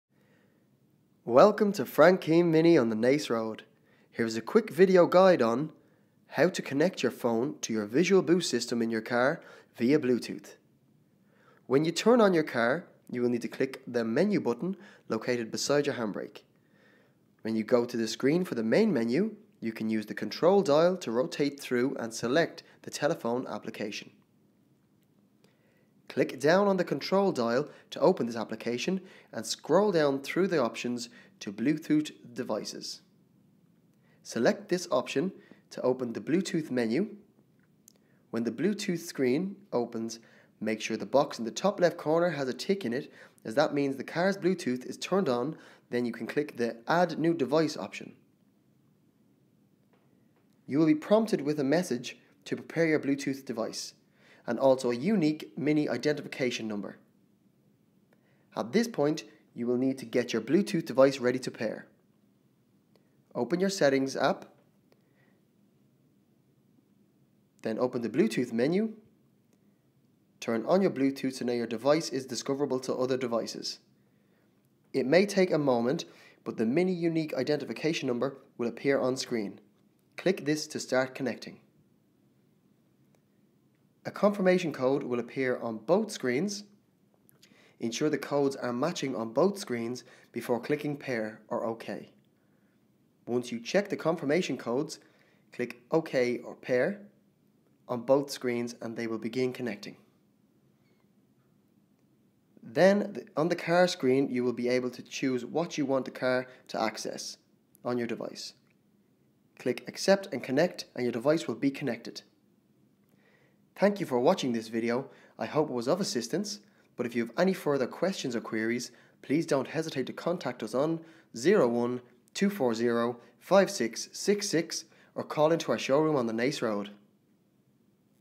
How to connect your phone to your visual boost system in your MINI
Resident MINI Genius, Dean Gibbons, demonstrates in the short film how to connect your phone to your visual boost system in your MINI via bluetooth.